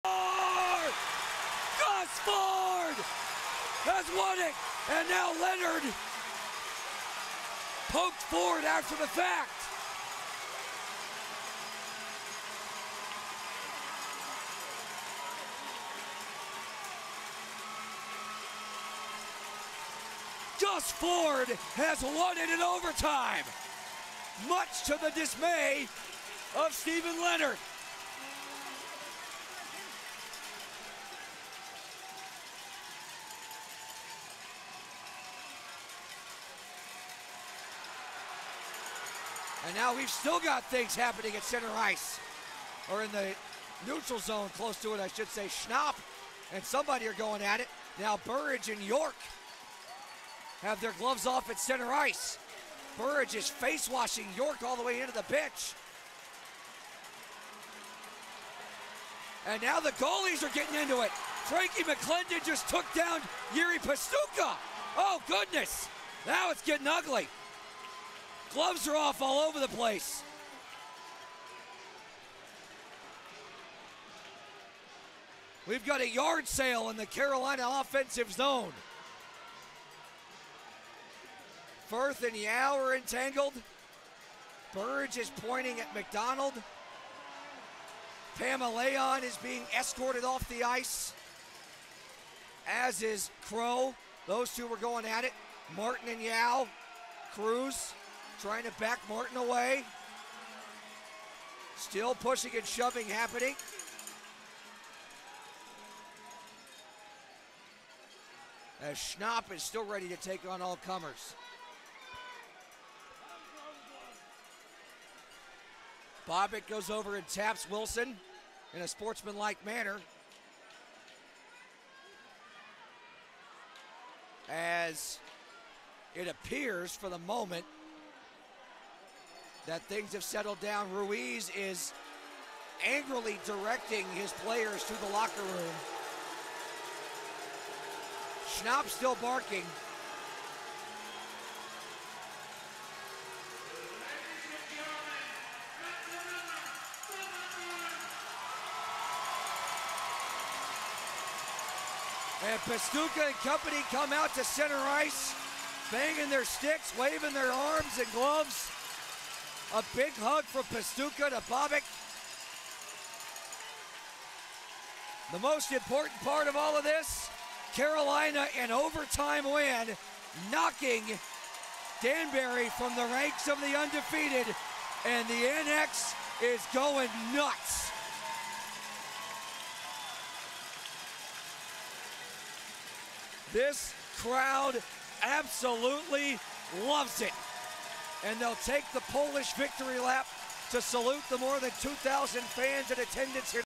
Bench Clearing Brawl. Federal Prospect Hockey League, Danbury Hatricks vs Carolina Thunderbirds.
You don't see this much, a good old fashion bench clearing brawl. Tempers over flowed after a hard fought battle between the FPHL Danbury Hat Tricks and the Carolina Thunderbirds. Carolina won it in OT and put an end to Danbury's 5 game winning streak.